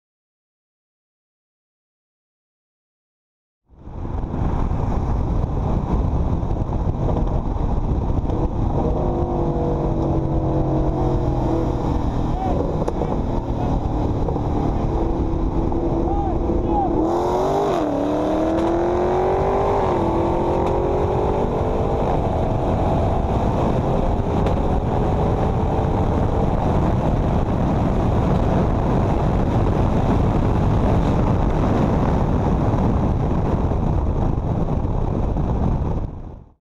Suburu WRX STI VS Dodge Challenger Hellcat Street Race
Check these bad boys out! Post comments below!

2015 Suburu WRX STI vs Dodge CHALLENGER HELLCAT 40 ROLL RACE

The STI has a stock location 30r turbo, Mahle Pistons, Stock Rods, Kelford 272 cams, ETS FMIC running 27lbs of boost on E85. (Boost Controlled Performance Tuned) "BCP" and made 494AWHP.

The Hellcat is pretty much stock besides a Bwoody CAI, Heat Exchanger, and Drag Radials. Filmed in Mexico.

@MoeThe Driver on IG, Twitter.

See you at WAM 3!